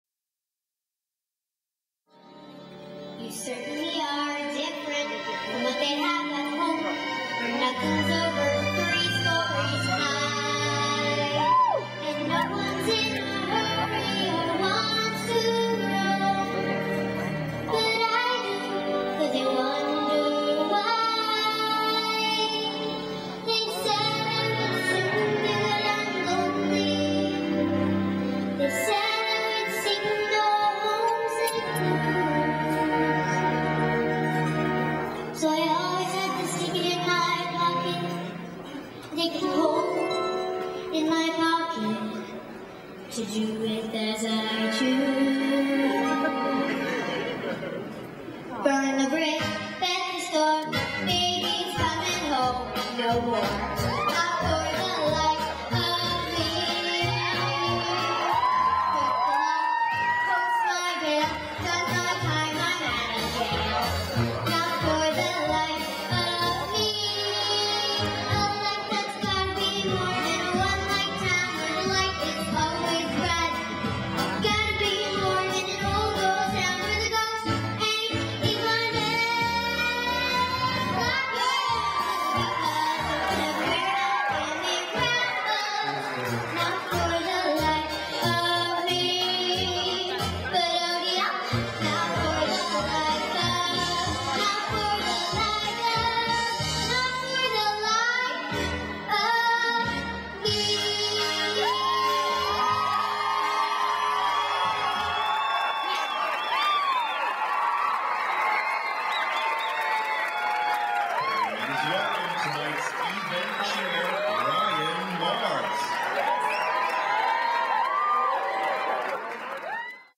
Hannah Knight sings during the Disneyland Gala 0:13
Hannah Knight, Vocalist...she is Fantastic! If it helps, my son Ross (the Yoyo Man) says she will be 11 in December.

Archive Filename: 121020 DisneyGala 0